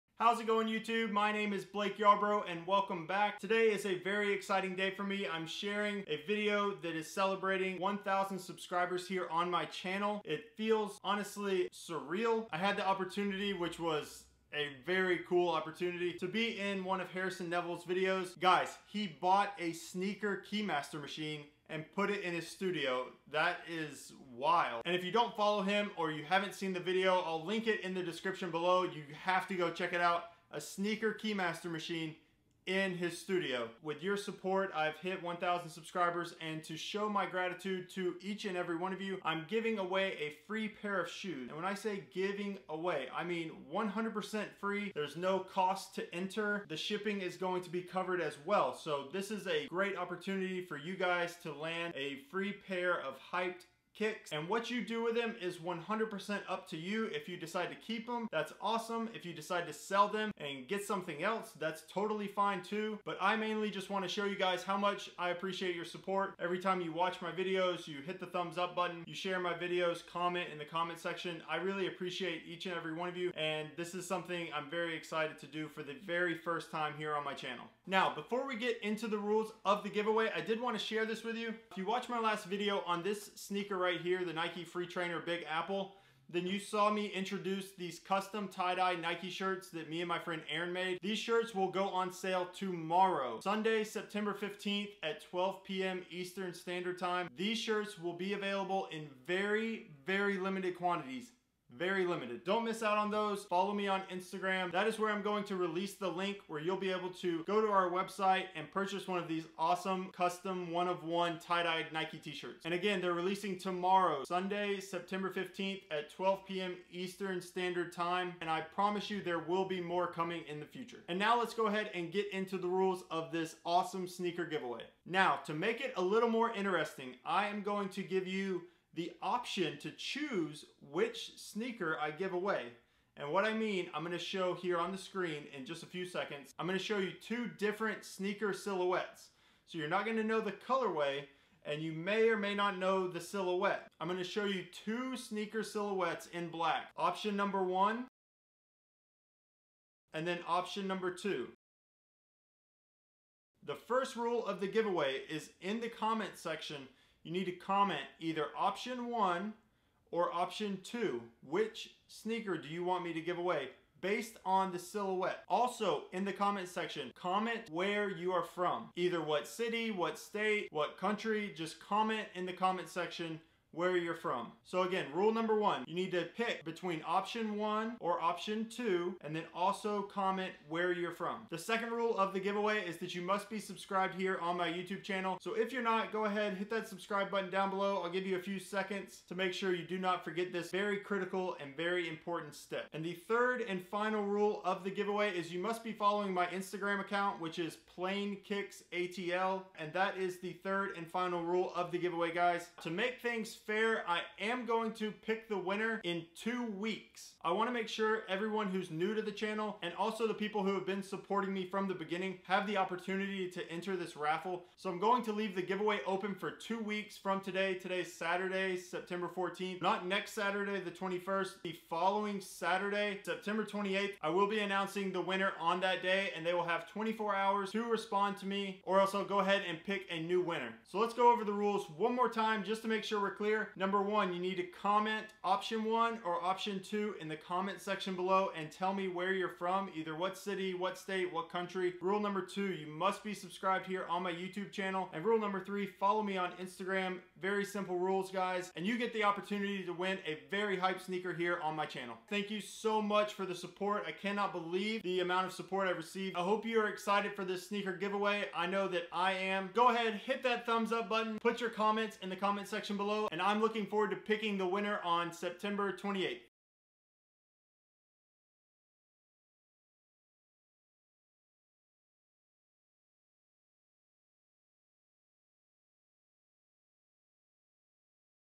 1,000th Subscriber Sneaker GIVEAWAY!!
After passing 1,000 subscribers on Thursday September 12, I am going to be giving away a hype pair of kicks!

TO ENTER:
1- Comment on this video which option YOU want (1 or 2) and where you are from.

That's it!

Winner will be chosen on September 28th.